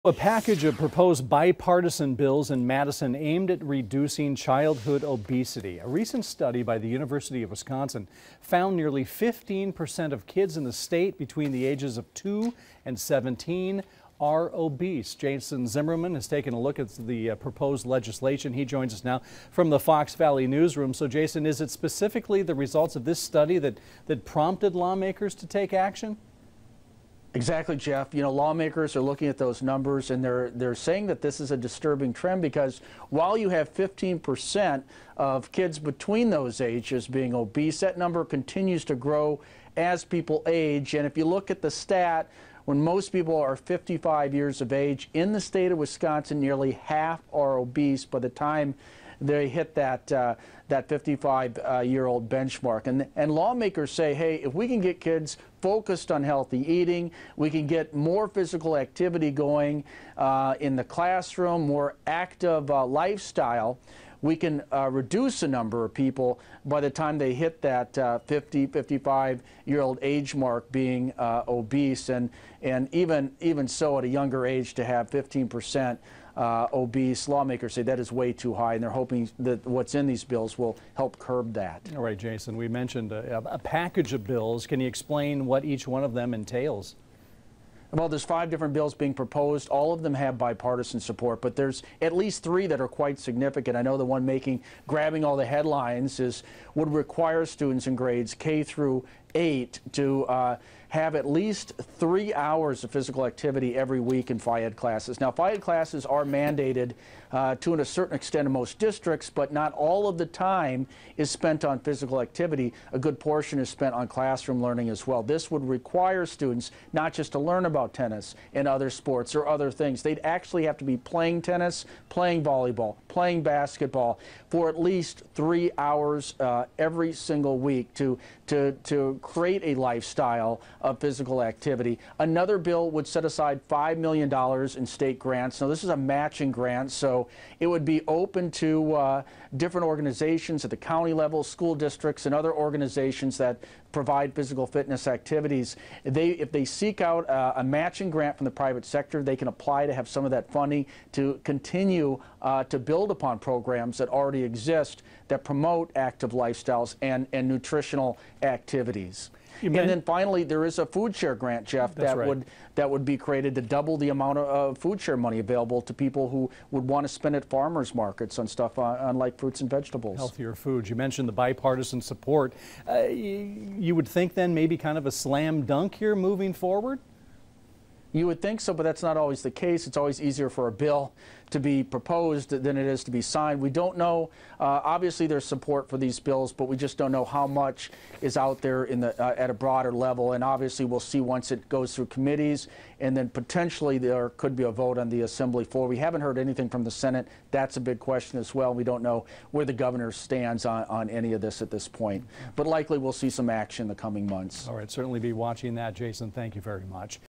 DEBRIEF: Childhood obesity legislation
Wisconsin lawmakers are looking to improve children’s health. How new legislation may help combat childhood obesity.